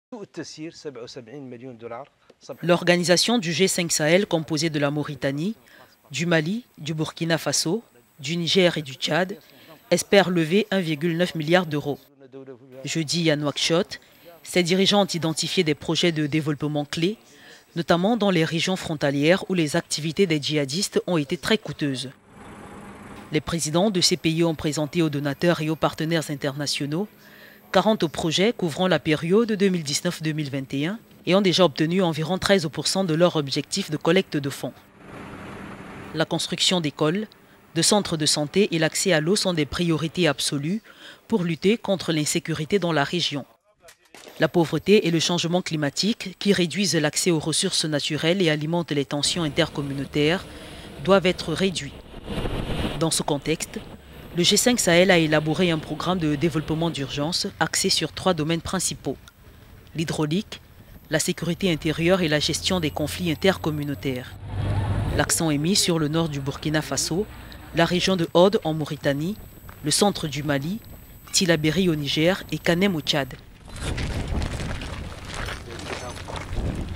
Sécurité et développement : le G5 Sahel en appelle aux bailleurs de fonds
*L'organisation G5 Sahel (Mauritanie, Mali, Burkina Faso, Niger et Tchad) espère lever 1,9 milliard d'euros jeudi pour des projets de développement, en particulier dans les régions frontalières où les jihadistes tirent parti des carences des États pour s'implanter.*

À Nouakchott, la capitale mauritanienne, siège du secrétariat du G5 Sahel, les présidents de ces pays, parmi les plus pauvres du monde, présentent 40 projets couvrant la période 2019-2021 devant bailleurs de fonds et partenaires in…